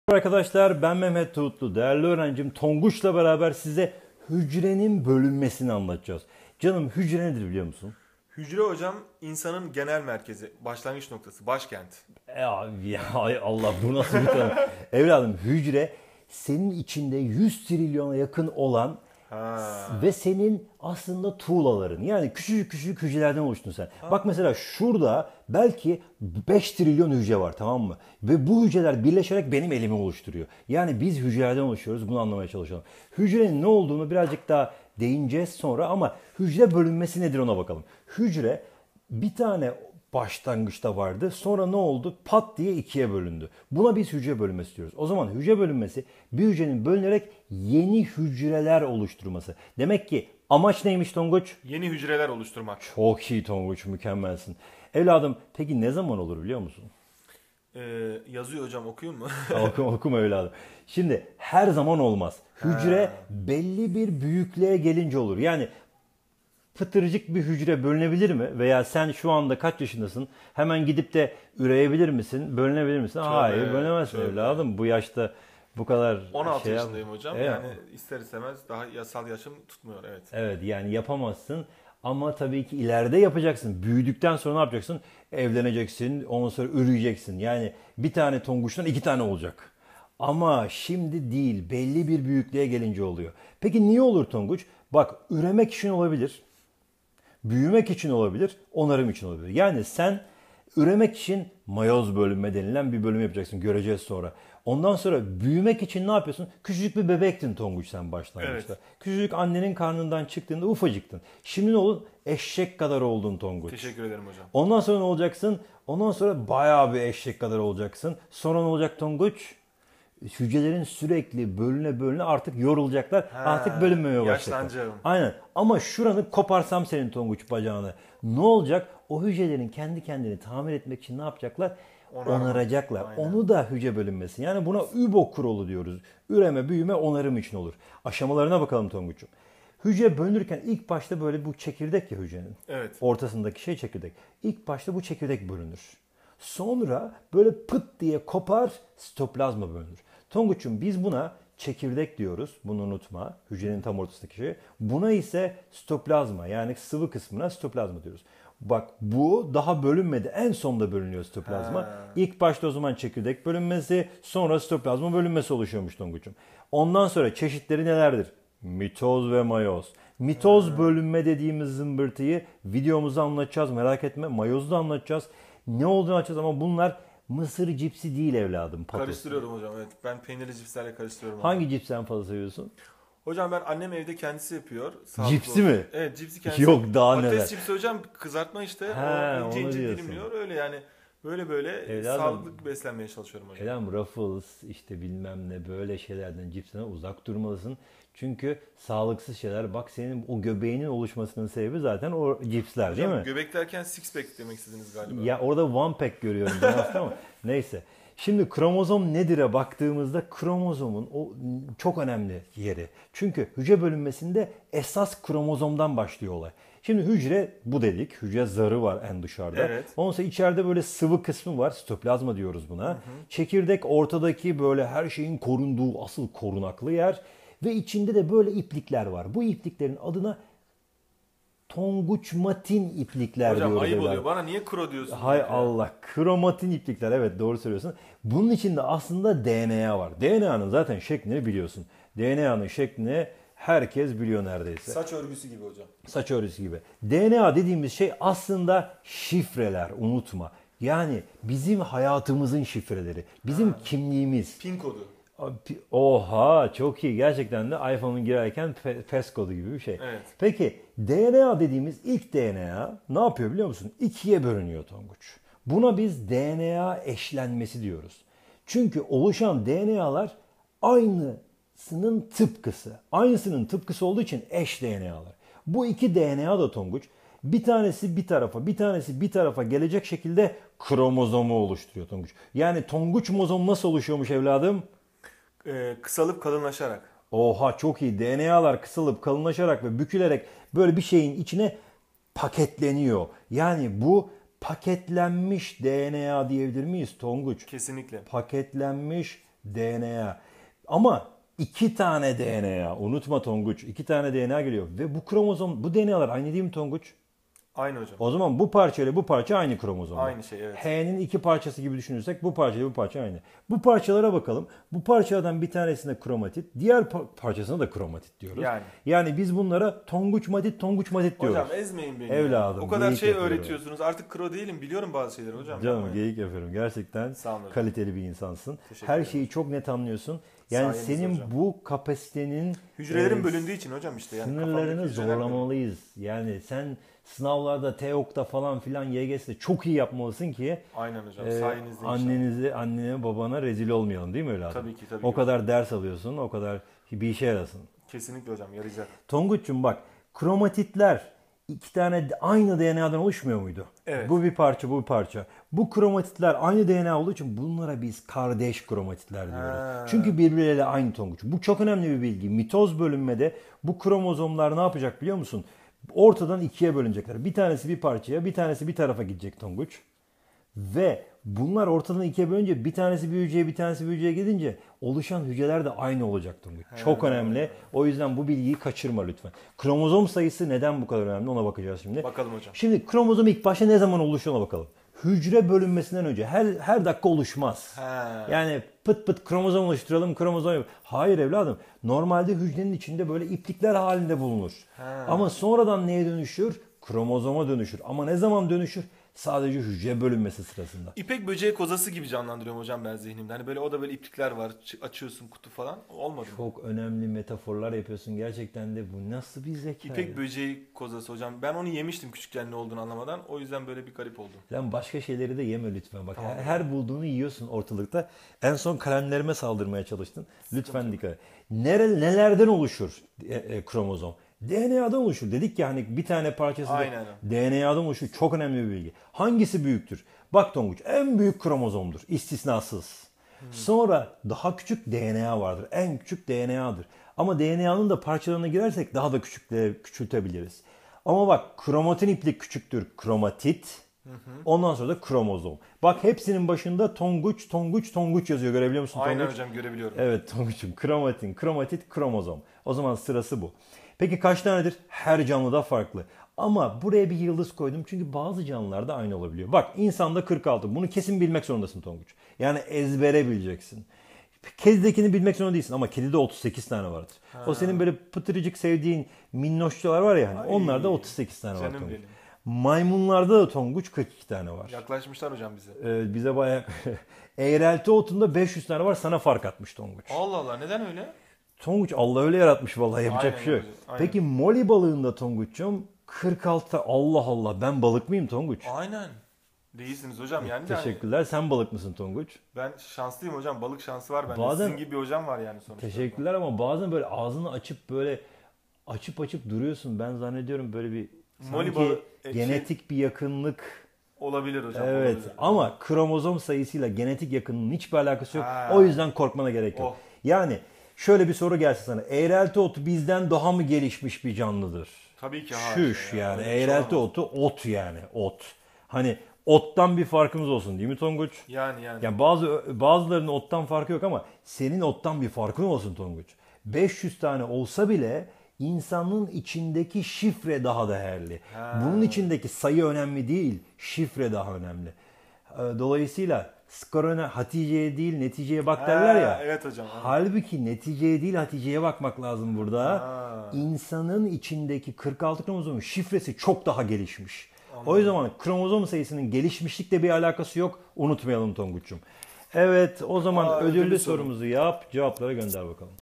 7.Sınıf Fen Bilimleri | Hücre Bölünmesi
7.sınıf Fen dersinde ''Hücre Bölünmesi'' konusunu 10 dakika'da sana öğretiyoruz.